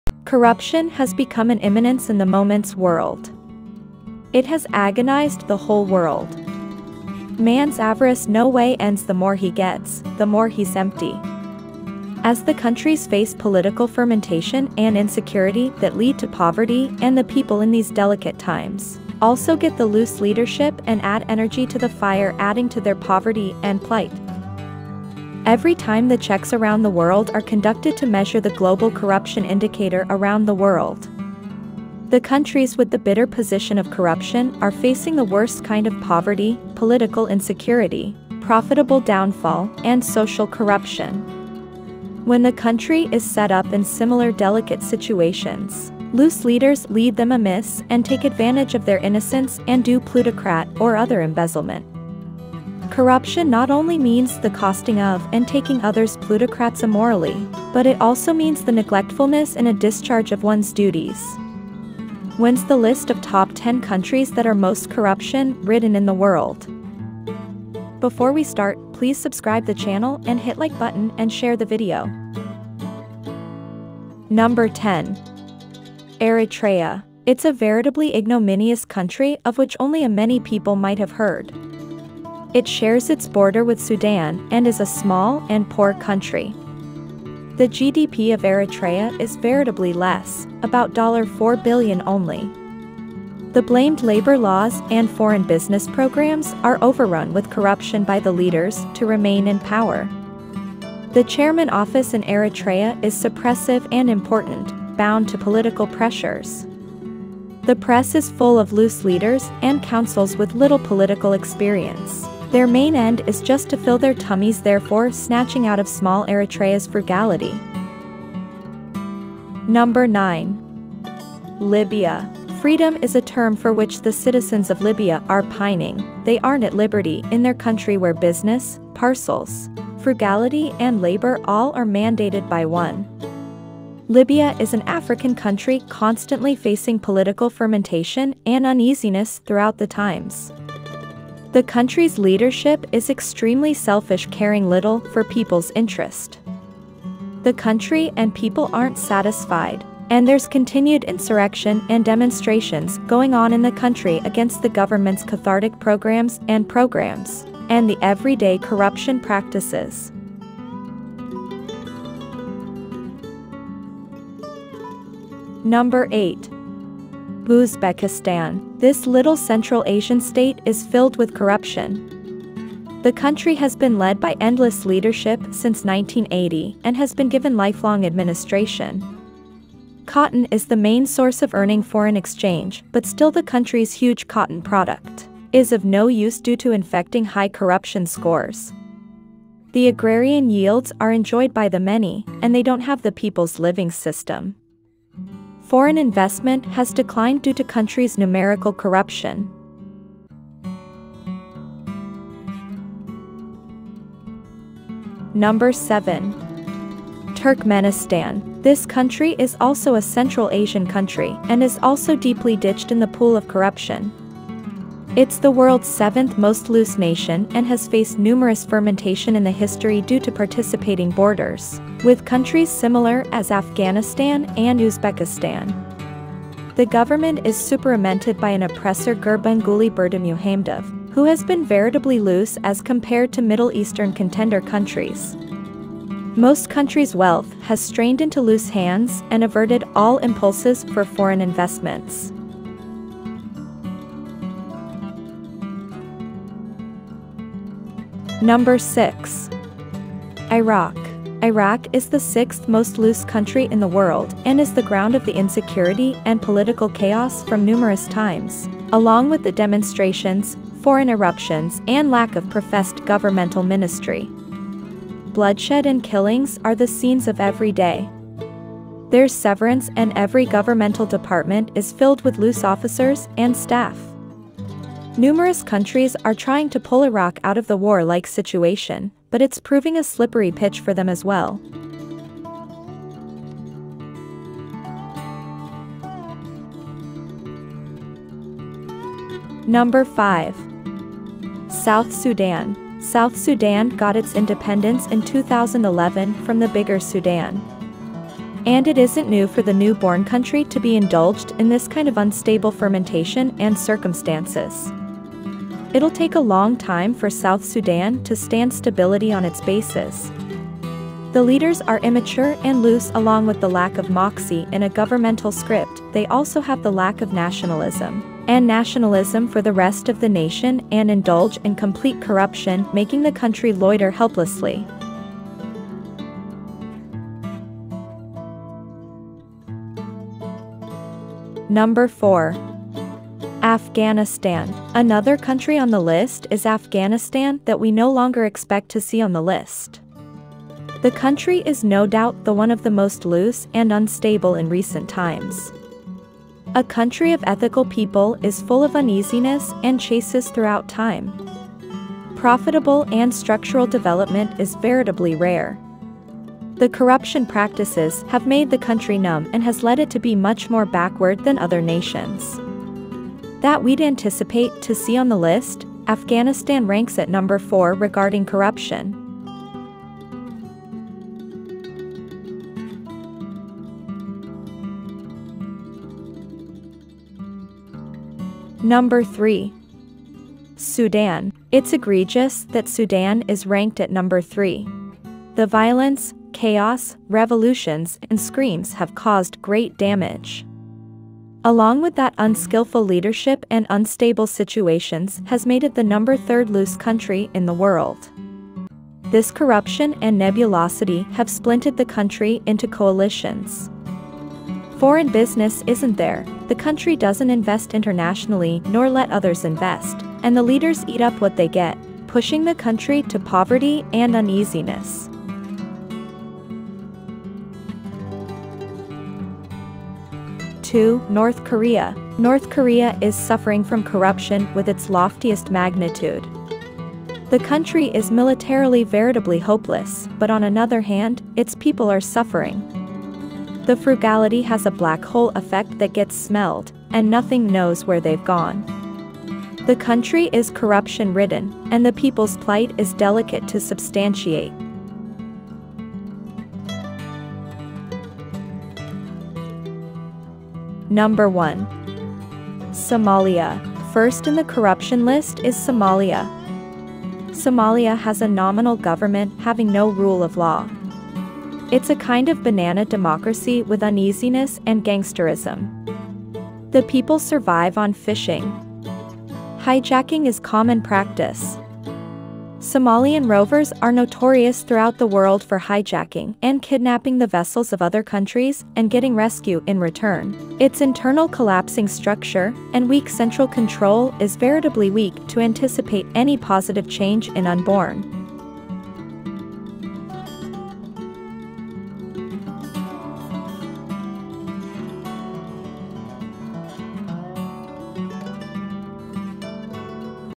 Top 10 Most Corrupt Countries List In The World - Travel Video Guide - Travel Croc
Corruption has become a menace in today’s world it has plagued the whole world countries. Man's Avarice never ends the more he gets, the more he is hungry. As the countries face political turmoil and instability that lead to poverty and the people in these difficult times also get the corrupt leadership and adds fuel to the fire increasing their poverty and plight.

Top 10 Most Corrupt Countries List In The World

 Every year the surveys around the world are conducted to measure the global corruption index around the world. The countries with the bitter level of corruption are facing worst kind of poverty, political instability, economic downfall, and social corruption. When the country is found in such difficult situations, corrupt leaders lead them astray and take benefit of their innocence and do money or other embezzlement. Corruption not only means the fetching of and taking others money illegally, but it also means the carelessness in a discharge of one’s duties. Here is the list of top ten countries that are most corruption-ridden in the world.

No 10. ERITREA
No 9. LIBYA
No 8. UZBEKISTAN
No 7. TURKMENISTAN
No 6. IRAQ
No 5. SOUTH SUDAN
No 4. ?
No 3. ?
No 2. ?
No 1. ?